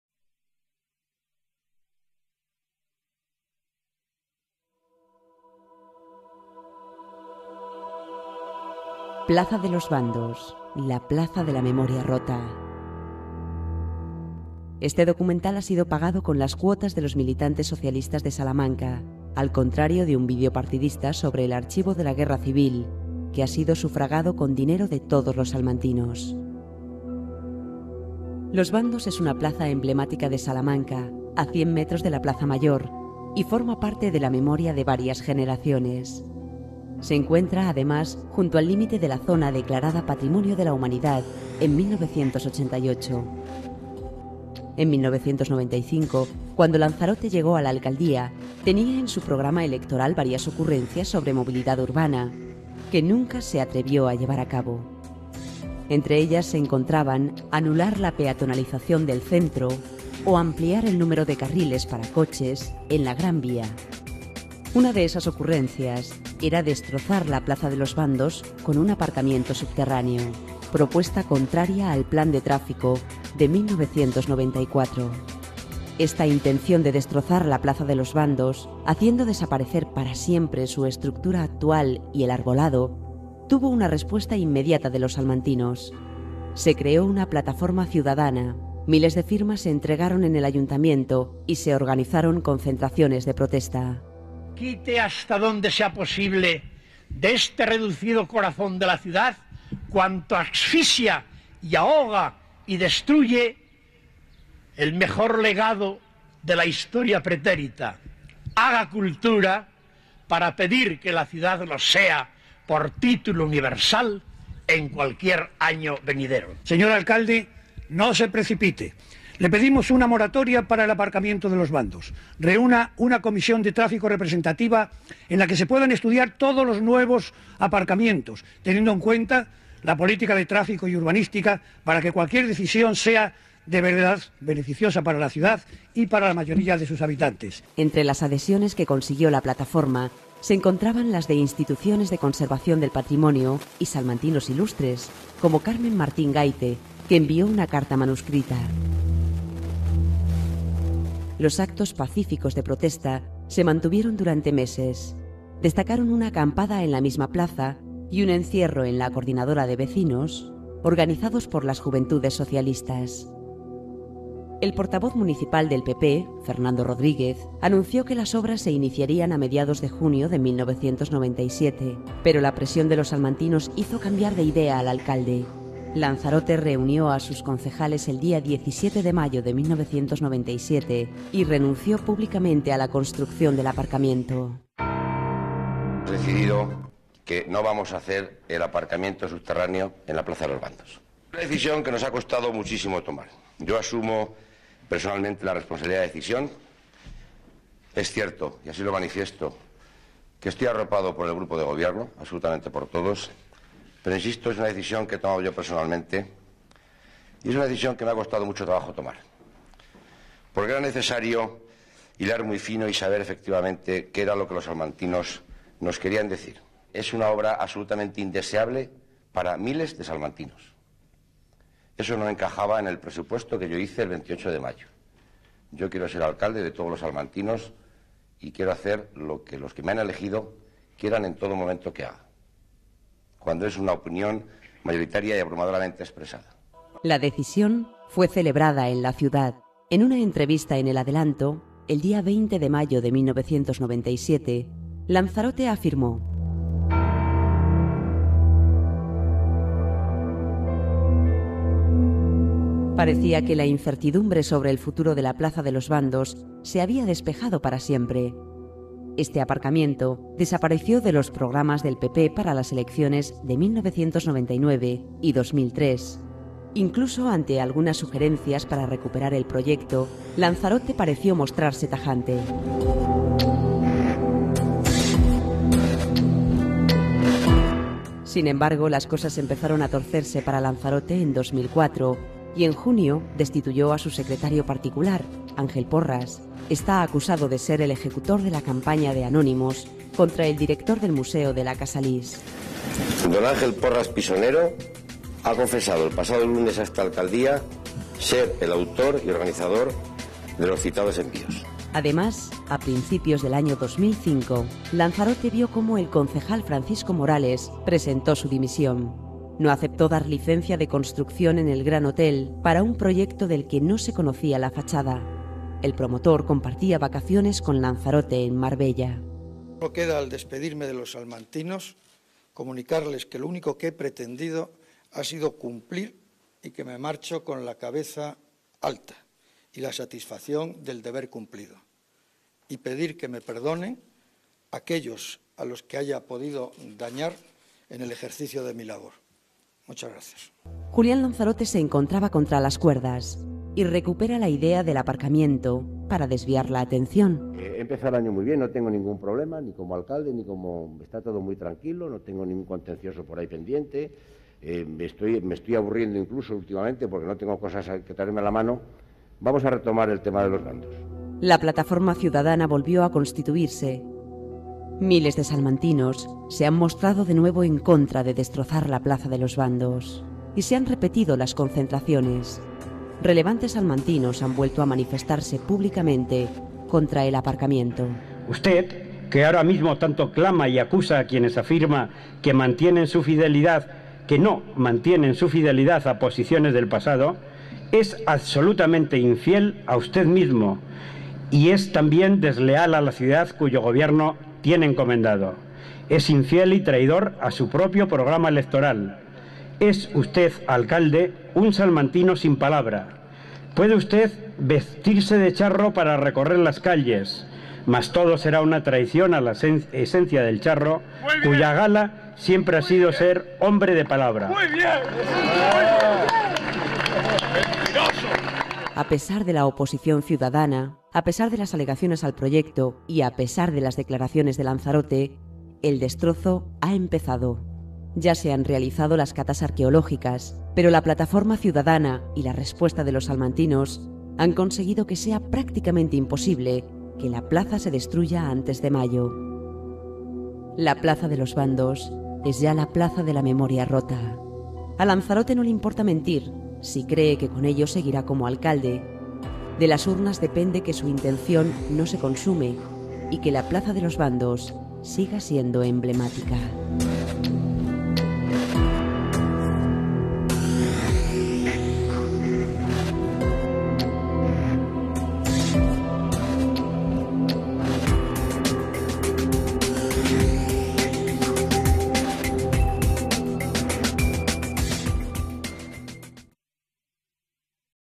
Plaza de los Bandos: La Plaza de la Memoria Rota
La tradicional Plaza de los Bandos de Salamanca está amenazada por la construcción de un aparcamiento subterráneo fuertemente rechazado por los ciudadanos.